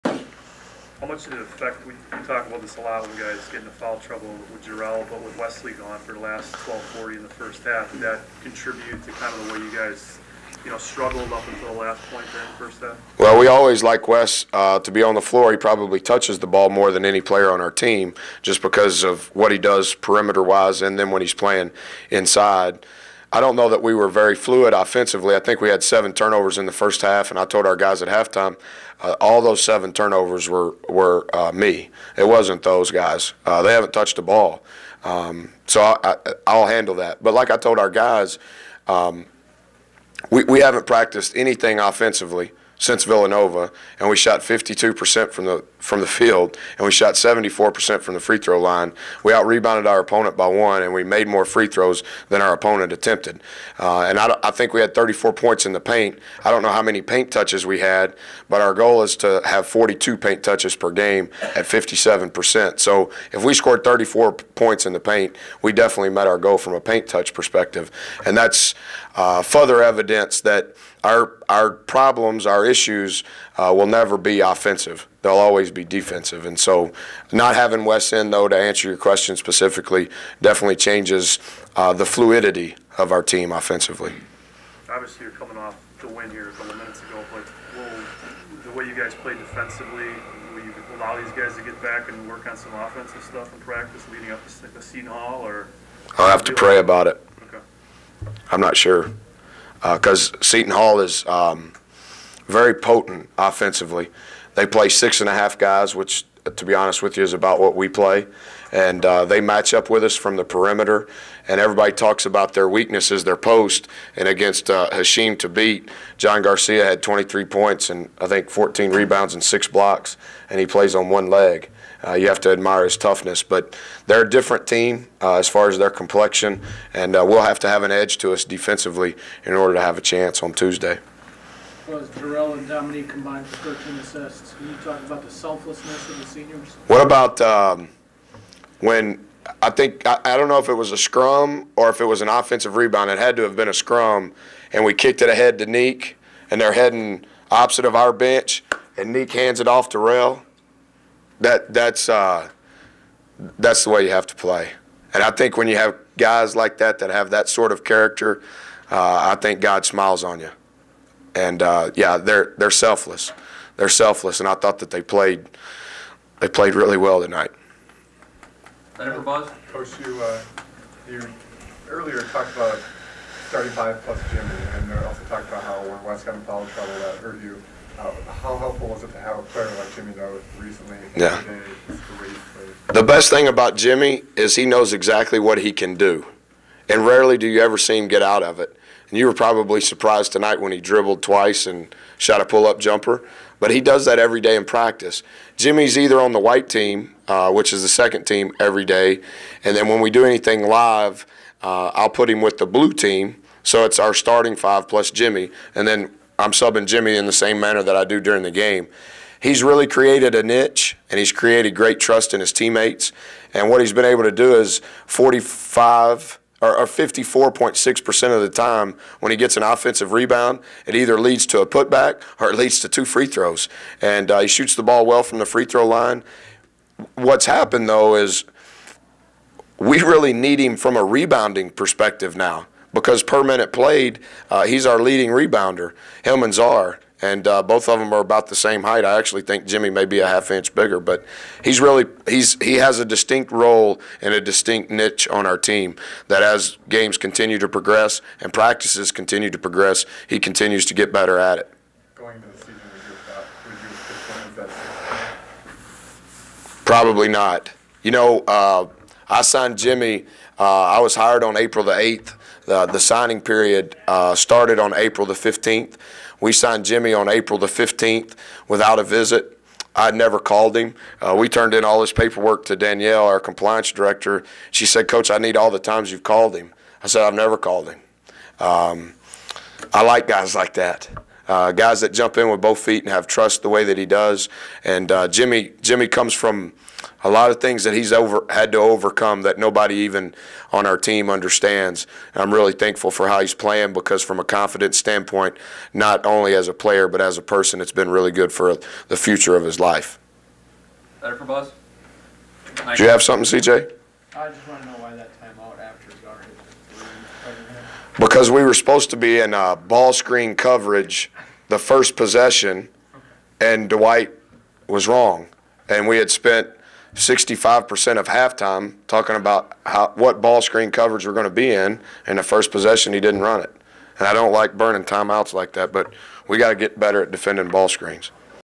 Buzz Williams: St. John's-MU PART 2 Press Conference 2-14-09
PART 2 of 2. Buzz Williams' postgame press conference after MU beats St. John's 73-59 at the Bradley Center on Valentine's Day, 2-14-09.